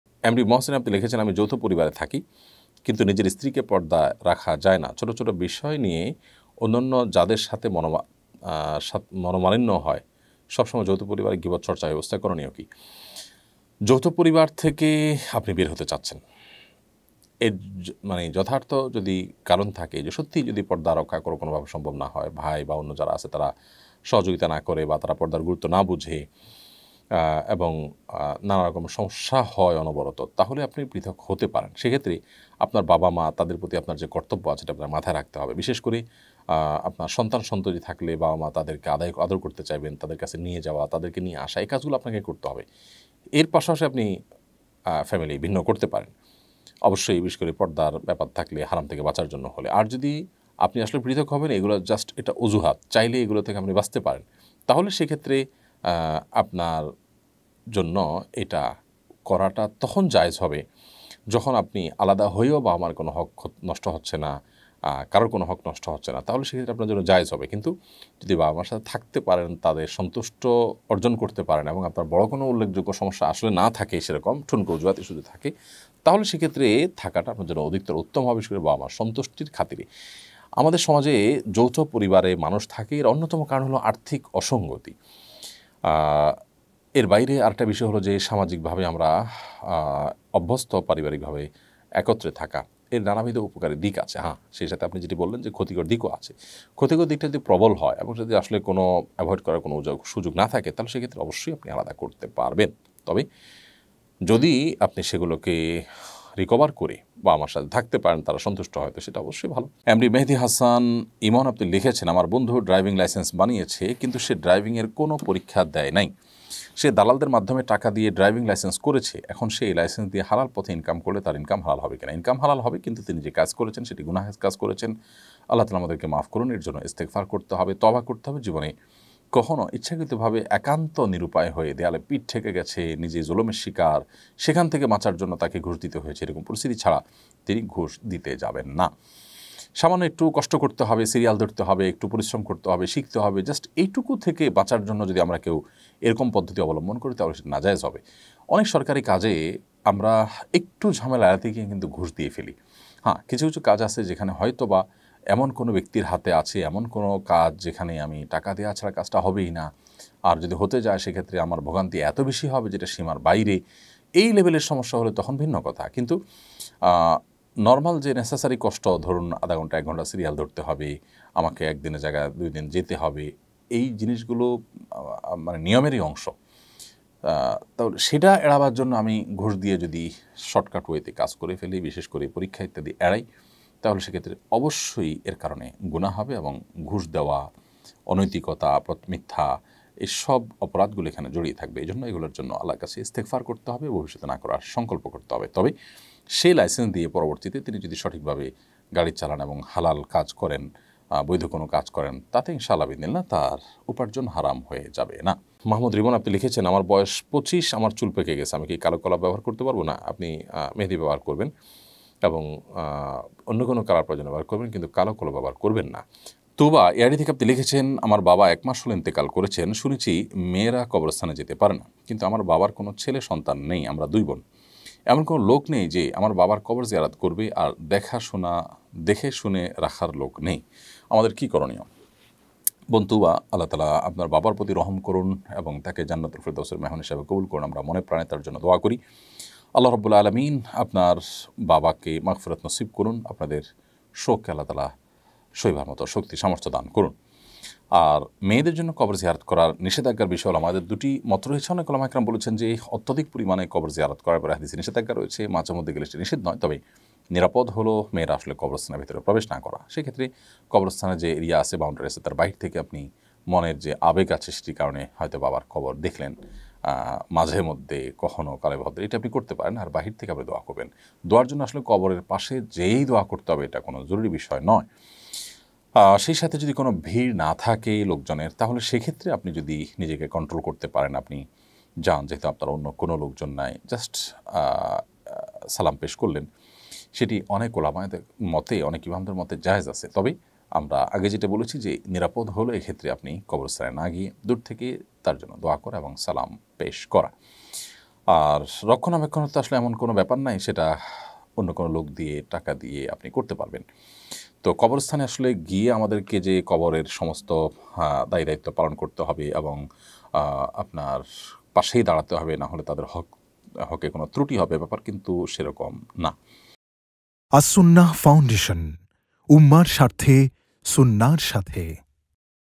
যৌথ পরিবারে স্ত্রীর পর্দা রক্ষা করা কঠিন;এমতাবস্থায় পৃথক হওয়া যাবে কি?
যৌথ পরিবারে স্ত্রীর পর্দা রক্ষা করা কঠিন;এমতাবস্থায় পৃথক হওয়া যাবে কি? new waz II waj II Sheikh ahmadullah